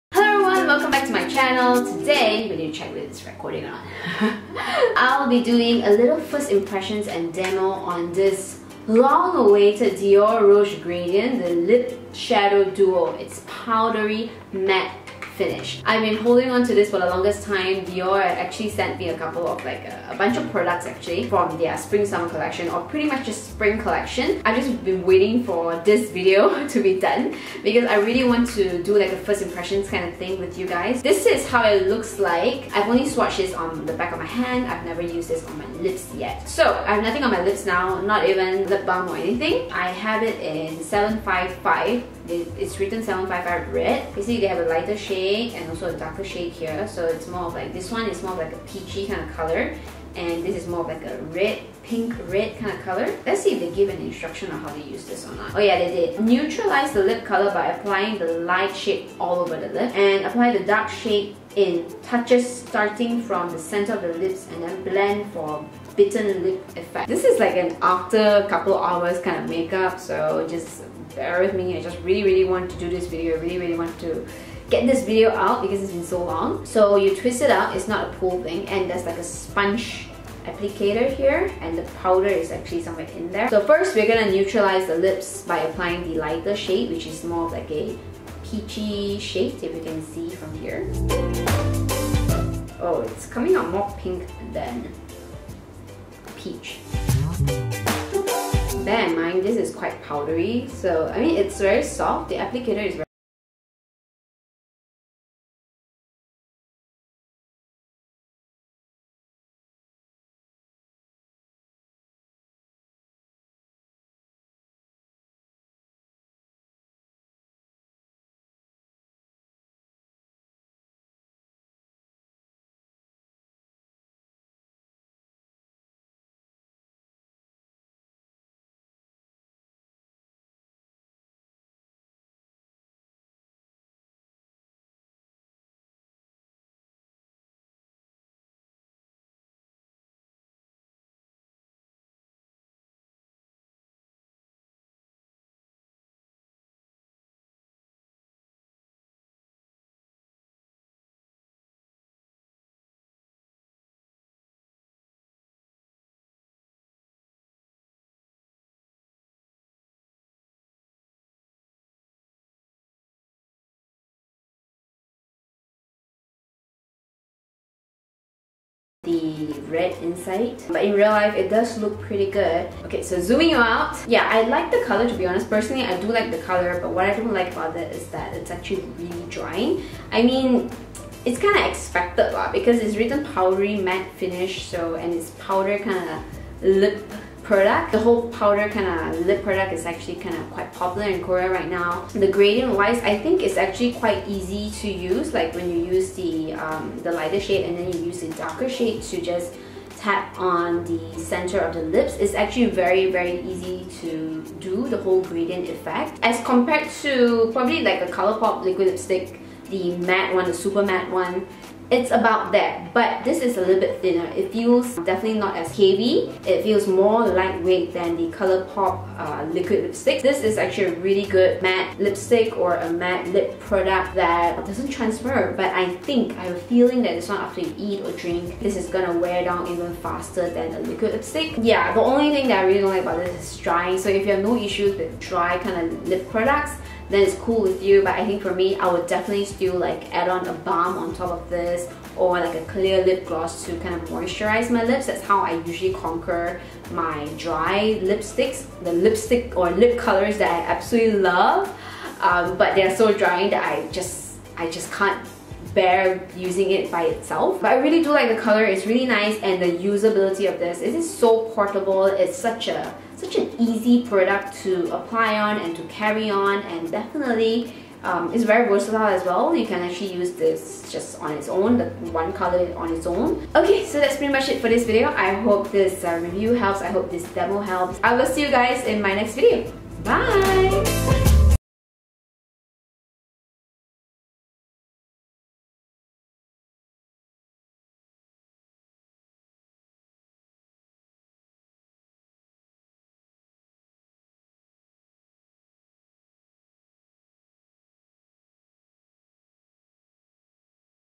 POWDER LIP PRODUCT?! DIOR Rouge Gradient Lip Shadow Duo \\ First Impressions \\ JQLeeJQ
These powder lippies are getting pretty trendy in Korea and Dior themselves came out with one. Is it worth it?

Keep Connected:
Instagram & Twitter: @jqleejq